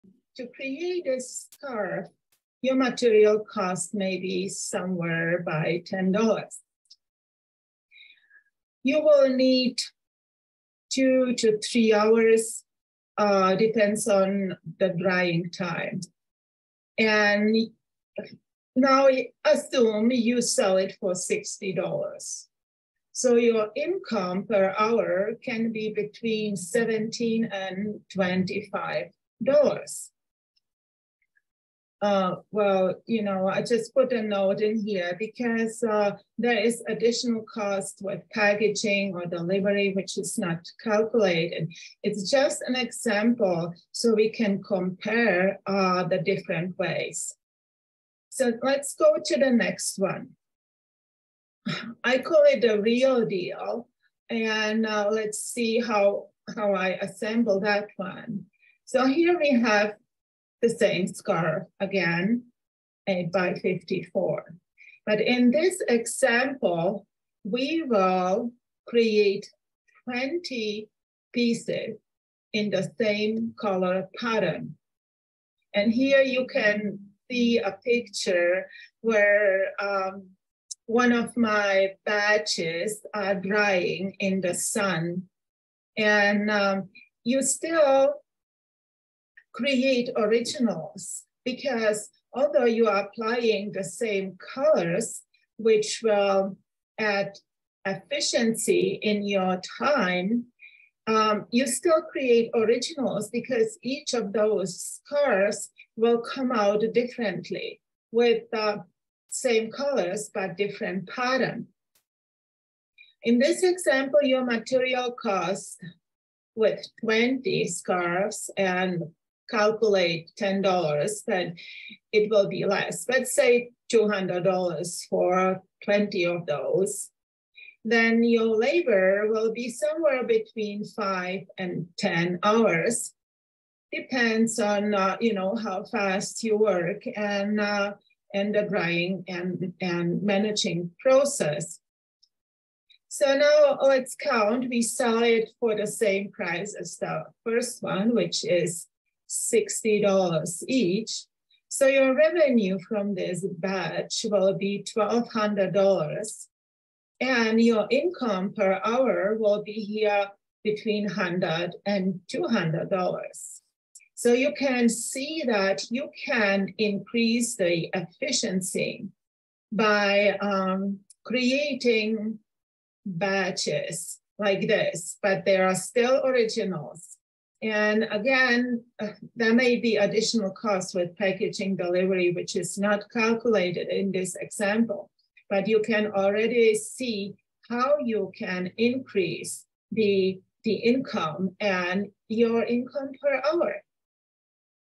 Free Event Online - Successful Silk Painter: Silk Painting For Income Generation
Attention all silk painters and creative souls ❤️! Want to monetize your creative passion? Join our 5-day online series and explore the three main ways to build income as an artist. Gain insights, ask questions, and choose your path to financial success. Limited spots available.
Registration is required!
How is it done? Learn free from my experience! Next week ✅ Online or in my learning studio